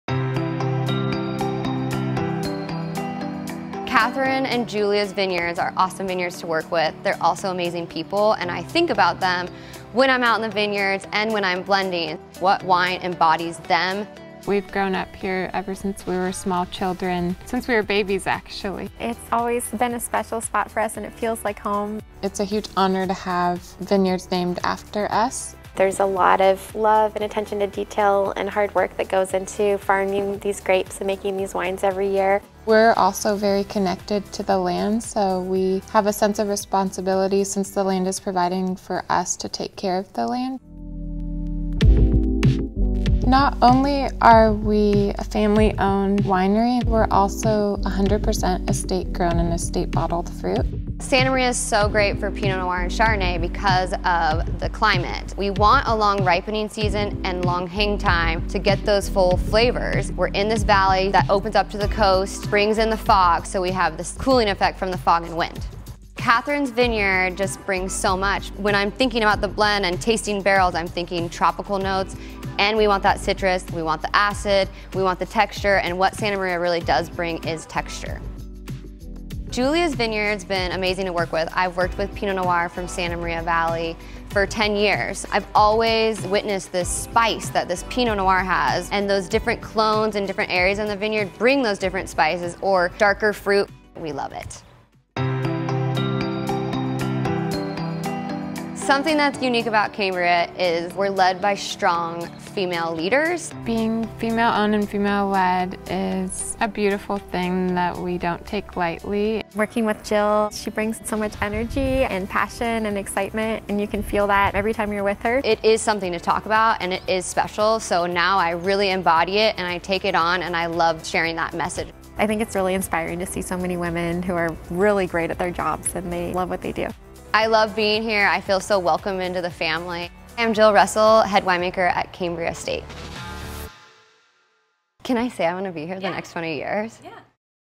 아름다운 그녀들의 아름다운 와인 | Cambria Sisters, Cambria Estate Winery
미국 최대 가족 와인 회사 잭슨 패밀리 이스테이트의 회장이자, 2013년 올해의 와인 인물로 선정된 최초의 여성 '바바라 뱅키'의 두 딸 캐서린 잭슨과 줄리아 잭슨 자매가 운영하는 아름다운 와이너리입니다.

우아한 피노 누아와 샤르도네 스타일을 만들어 내죠.